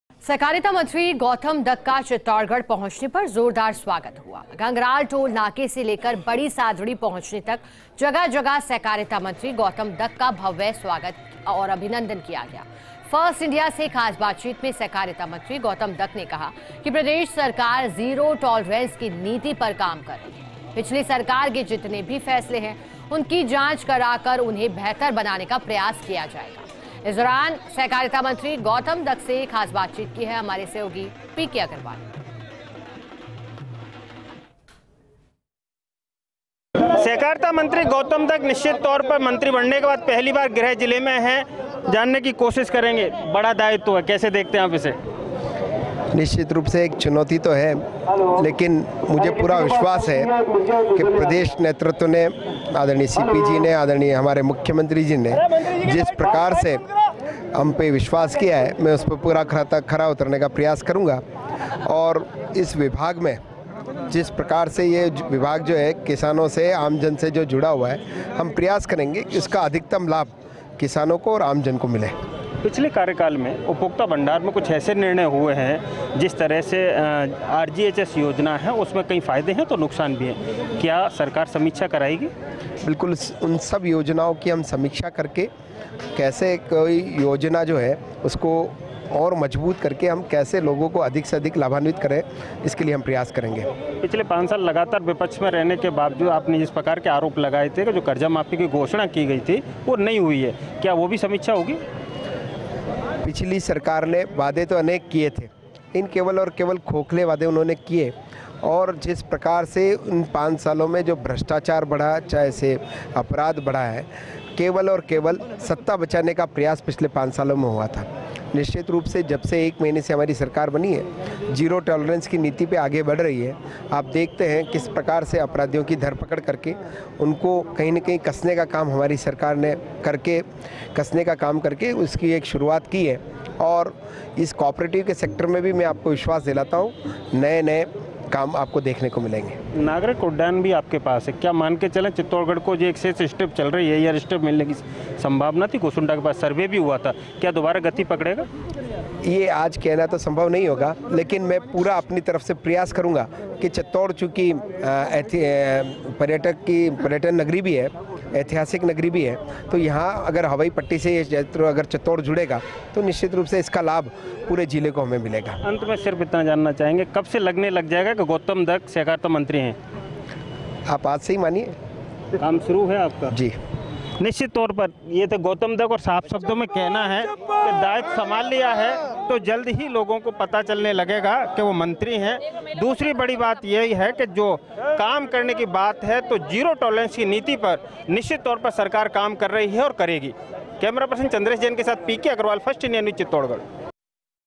जीरो टॉलरेंस की नीति पर काम कर रही है भजनलाल सरकार: Gautam Dak | Rajasthan Government
About this Video:
जीरो टॉलरेंस की नीति पर काम कर रही है भजनलाल सरकार: Gautam Dak | Rajasthan Government  | 06 January 2024